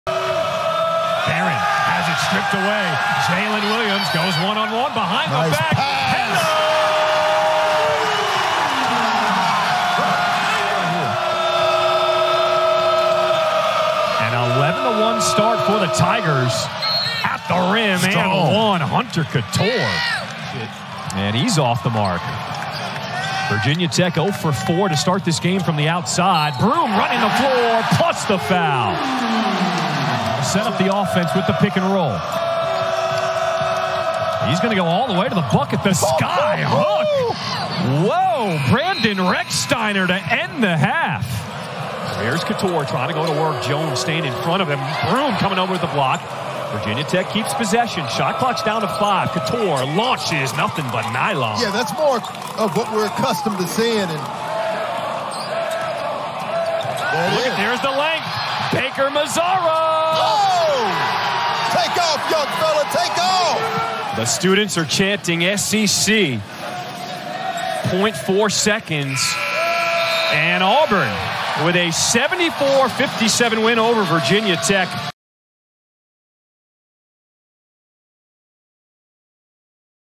Virginia Tech vs. Auburn Game Highlights | 2023-24 Men's Basketball Highlights
Virginia Tech vs. Auburn: The Virginia Tech Hokies traveled to Auburn and came up short on the road, 74-57. Hunter Cattoor registered 16 points and 6 rebounds for VT. John Camden and Jaydon Young each scored 9 points for the Hokies.